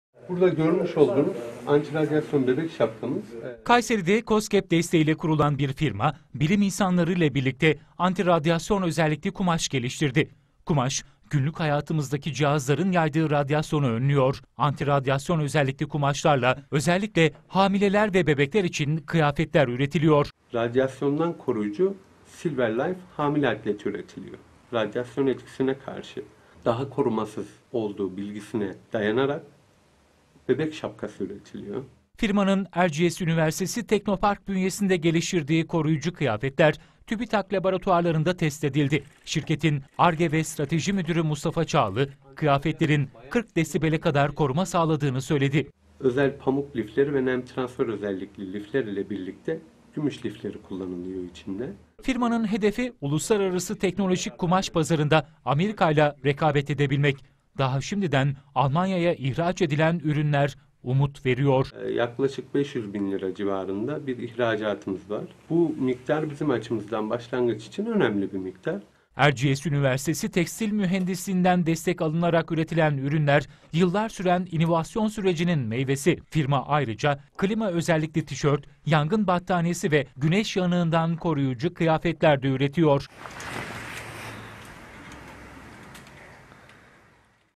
Radyasyonun etkilerini azaltan kumaş geliştirildi
Kayseri'de bilim insanları ve bir özel firma tarafından radyasyonun etkilerini azaltan kumaş geliştirildi. Hamileleri ve bebekleri radyasyondan koruyan kıyafetler de üretiliyor ve Almanya'ya ihraç ediliyor. Hedef, uluslararası pazarda ABD teknolojisi ile rekabet edebilmek.